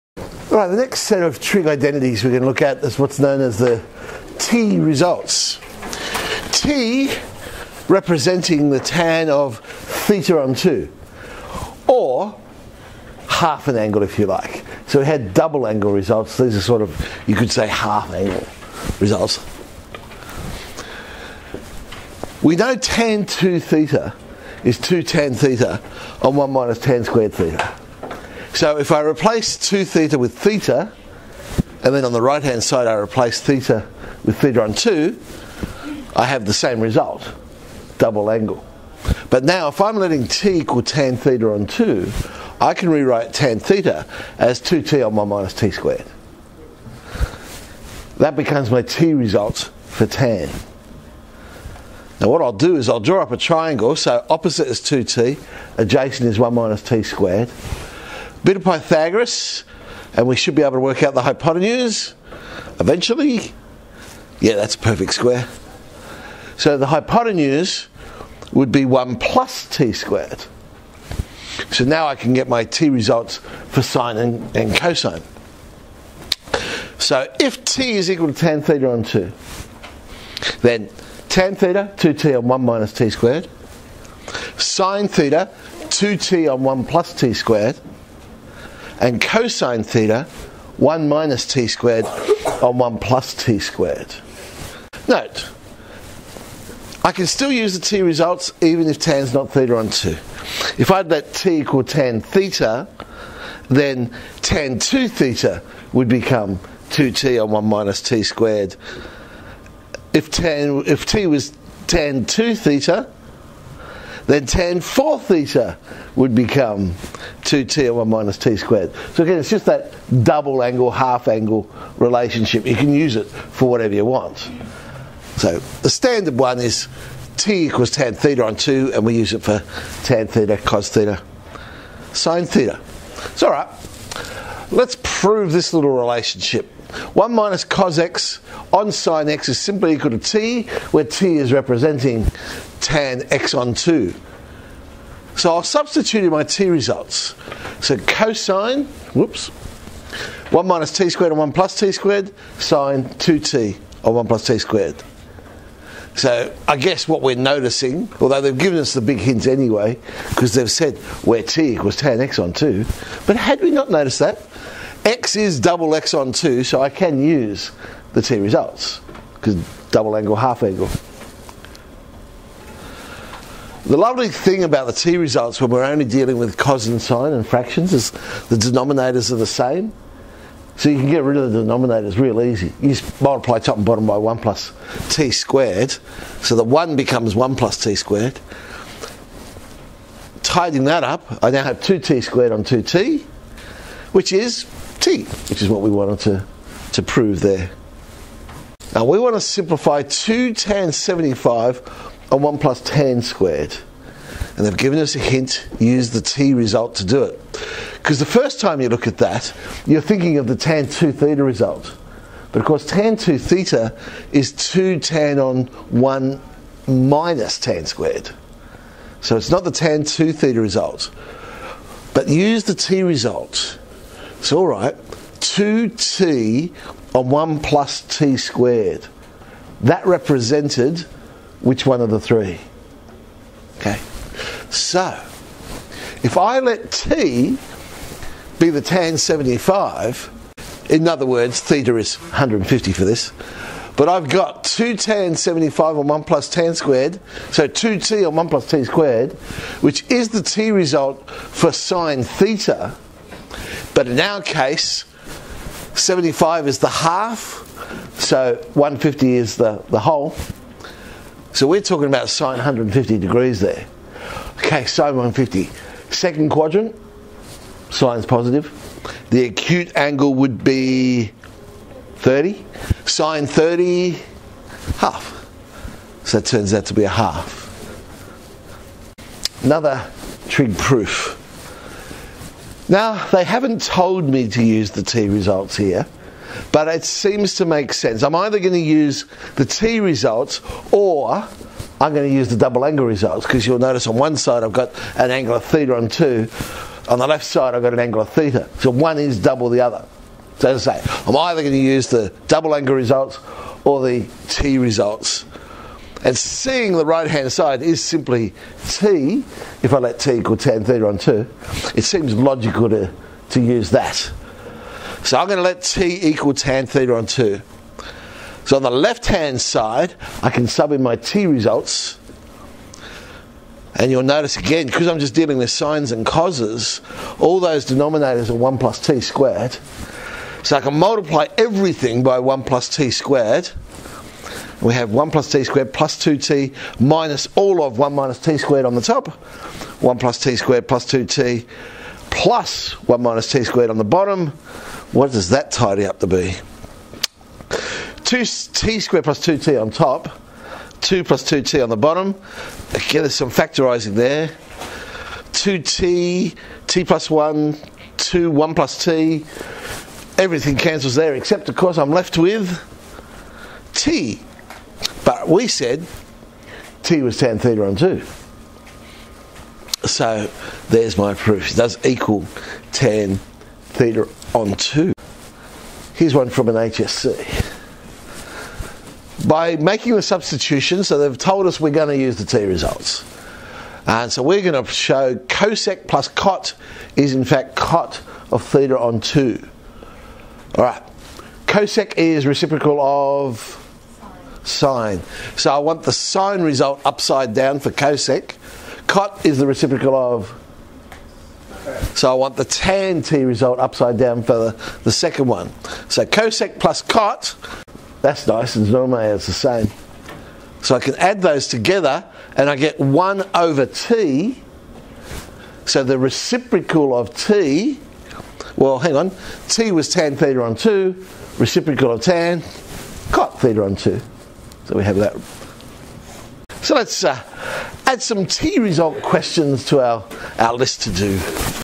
11X1 T08 05 t results 2017
Turning trigonometry into algebraic fractions vis the t-results